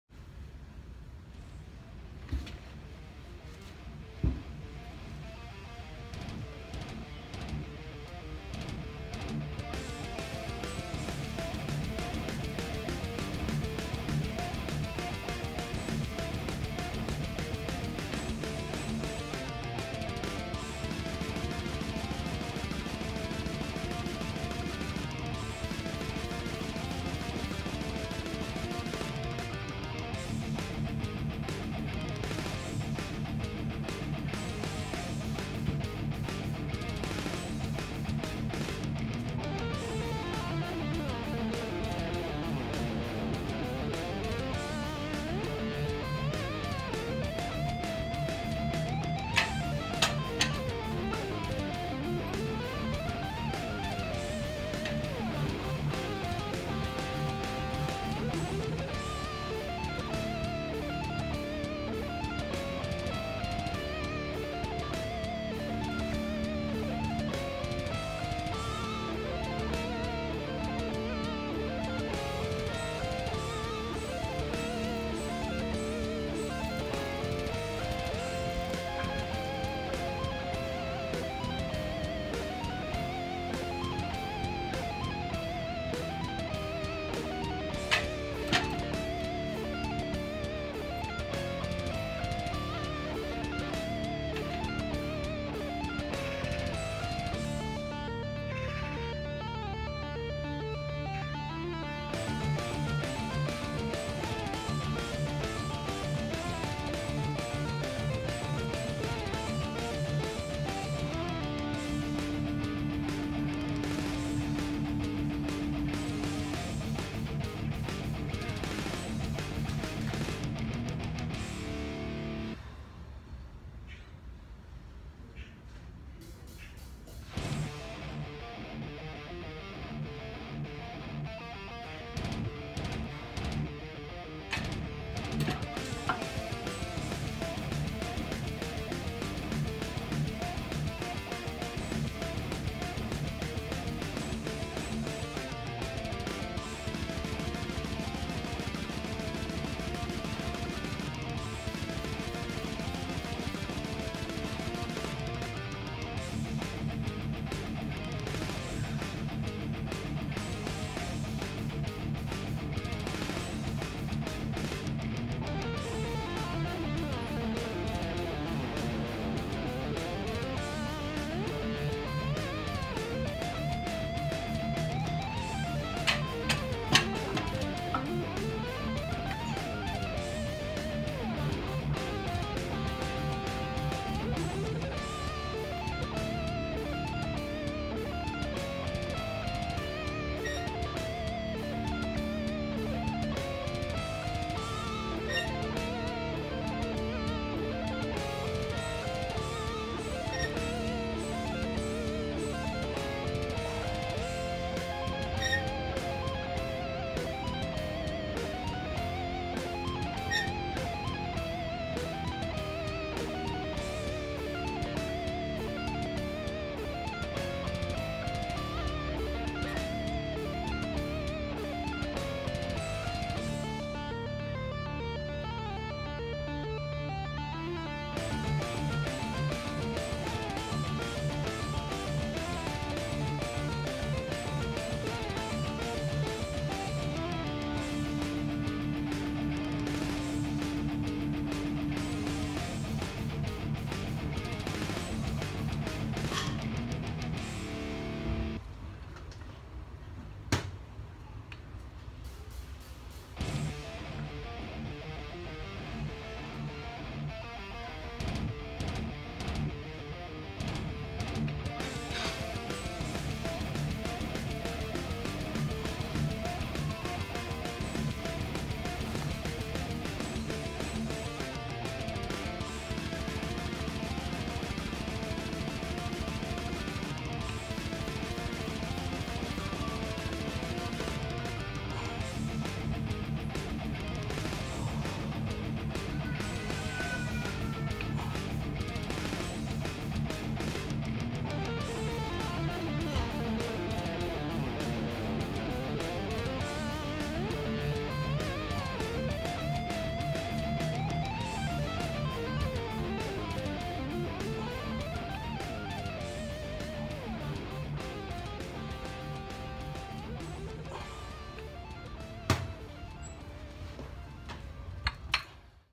Benchpress and Biceps Curls Workout in the Garden
Benchpress 60kg: 3x10 + 1x6 with 2 min rest

Biceps Curls 30kg: 1x25